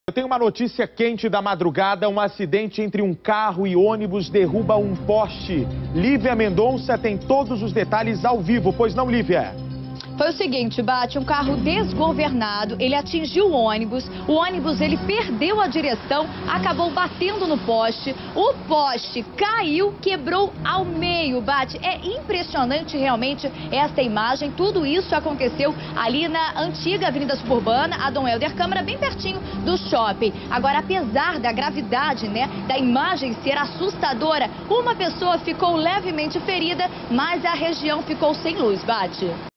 Carro bate em ônibus que derruba poste na zona norte do Rio
Um carro desgovernado atingiu um ônibus, o motorista do coletivo perdeu a direção e bateu em um poste que caiu e quebrou ao meio. O acidente aconteceu na avenida Dom Hélder Câmara, na zona norte do Rio.